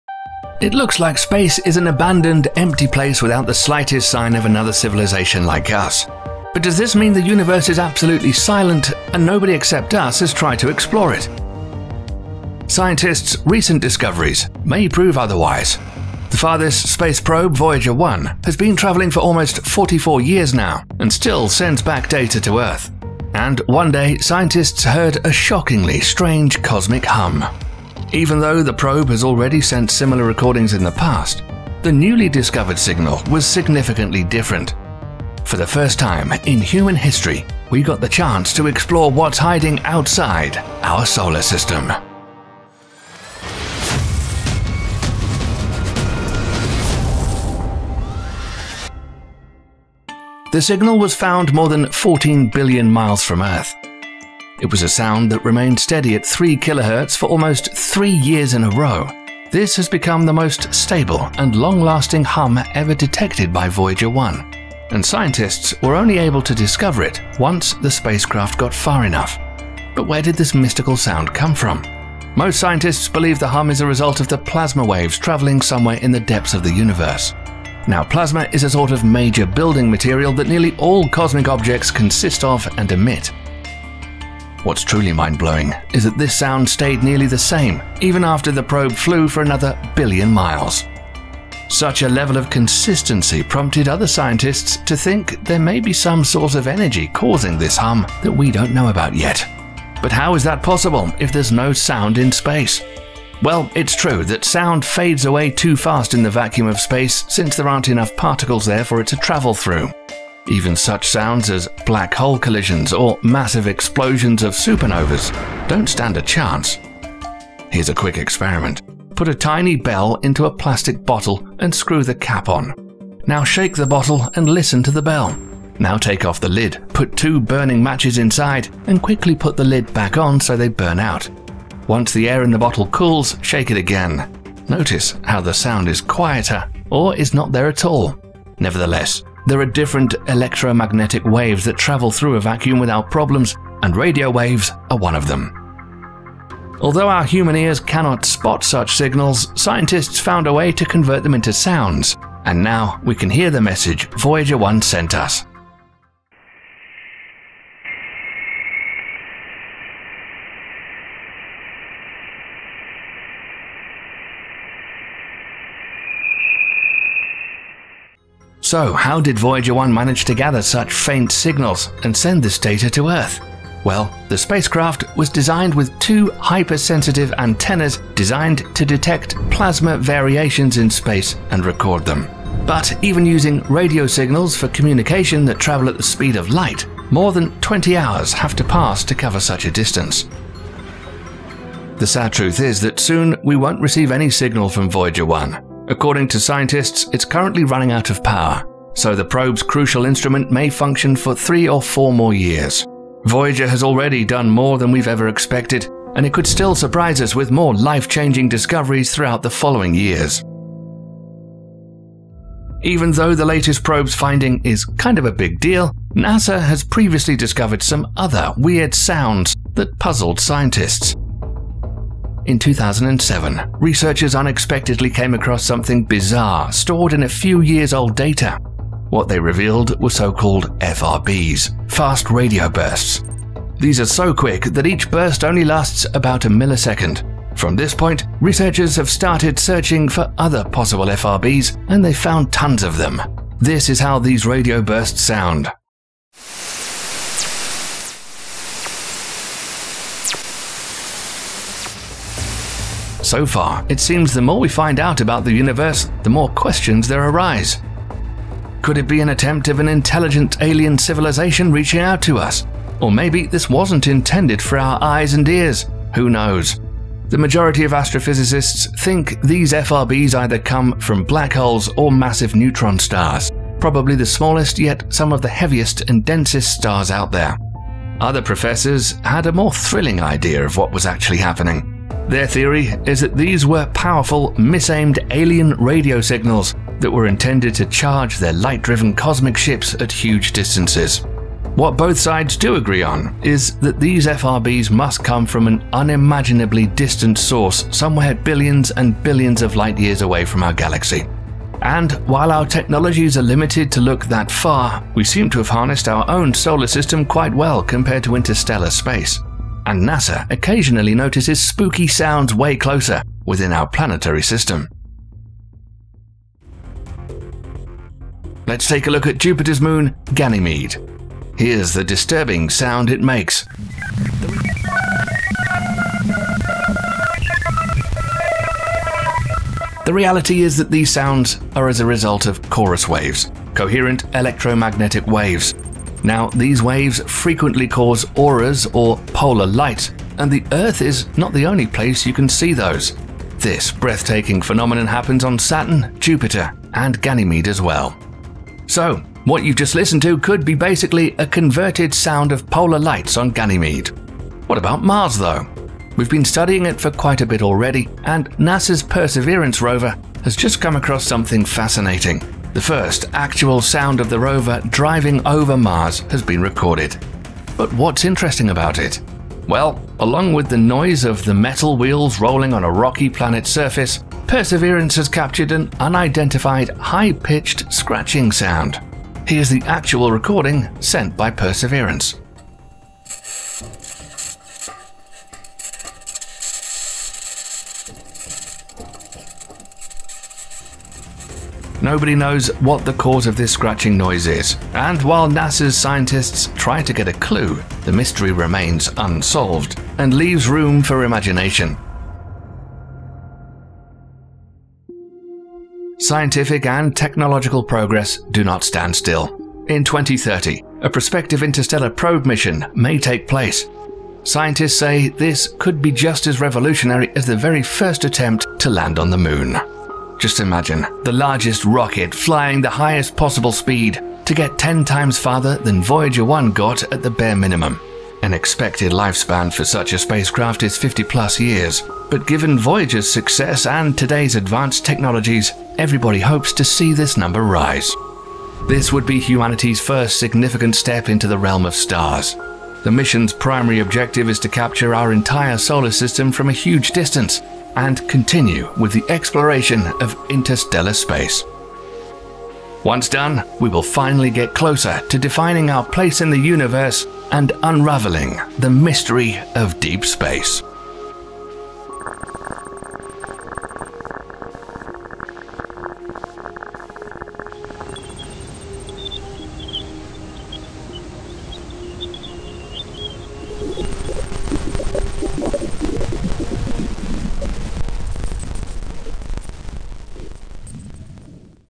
Voyager 2 Comes Back Online And Detects A Mysterious Hum
It looks like space is an abandoned empty place without the slightest sign of another civilization like us. But does this mean the universe is absolutely silent and nobody except us has tried to explore it? Scientists’ recent discoveries may prove otherwise.

The farthest space probe Voyager 1 has been traveling for almost 44 years now and still sends data back to Earth. And one day, scientists heard a shockingly strange cosmic hum.

Even though the probe has already sent similar recordings in the past, the newly discovered signal was significantly different. For the first time in human history, we got the chance to explore what’s hiding outside our solar system!